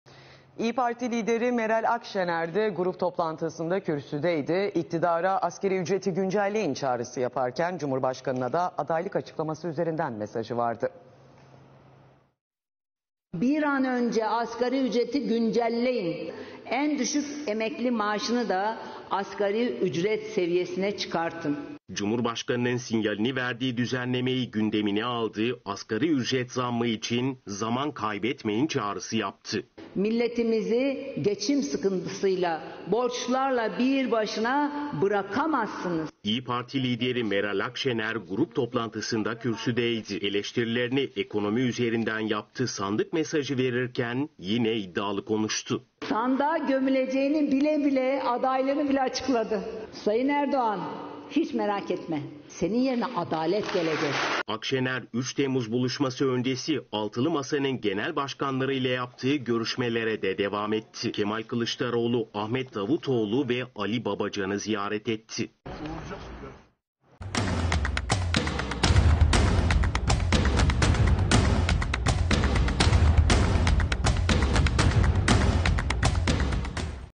Akşener'den asgari ücret çıkışı!
İYİ PARTİ LİDERİ MERAL AKŞENER DE GRUP TOPLANTISINDA KÜRSÜDEYDİ. İKTİDARA "ASGARİ ÜCRETİ GÜNCELLEYİN" ÇAĞRISI YAPARKEN, CUMHURBAŞKANI'NA DA ADAYLIK AÇIKLAMASI ÜZERİNDEN MESAJI VARDI.